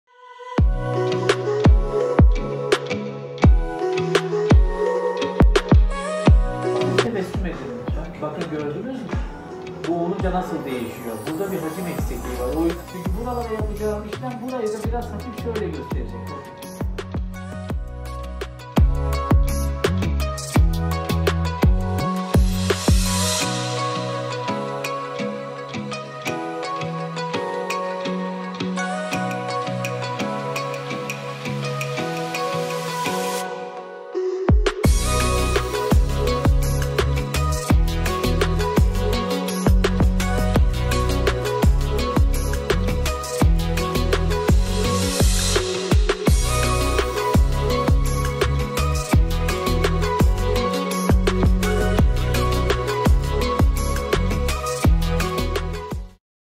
GENÇ VE DOLGUN YANAKLAR • Dr. Hasan Fındık
Sevgili hastama, Nasolabial ve Elmacık Dolgusu yaptım👨‍⚕️
❣️ Burun- dudak arası çizgisi yani gülme çizgimiz anlamına gelen, Nasolabial dolgusu yüz bölgesindeki kırışıklıklar için enjeksiyon yöntemi ile cilde uygulanan bir tür tedavi yöntemidir. Nasolabial dolgu uygulaması yapılan bölgede ince çizgiler ve derin kırışıklıklar daha düz bir görünüme kavuşur.
❣️ Kişide yapısal ya da zayıflığa bağlı olarak elmacık kemiği çıkık ya da çene hattı belirgin değilse veya kişide yaşın ilerlemesinden dolayı elmacık kemiği bölgesindeki hacim azalmaya başladıysa, kişiye standart estetik ölçülerine sahip olması için sıkça tercih edilen bir yöntem elmacık dolgusudur.
❣️Her iki dolgu içinde cilde enjekte edilen sıvı, yaşa bağlı olarak miktarı azalan kollajen üretimini tetikler, genç ve dolgun bir cilt sağlar. 🌹
Bilgi ve ücretsiz görüşme için yoruma ‘❤️’ bırakmanız yeterli ✍🏻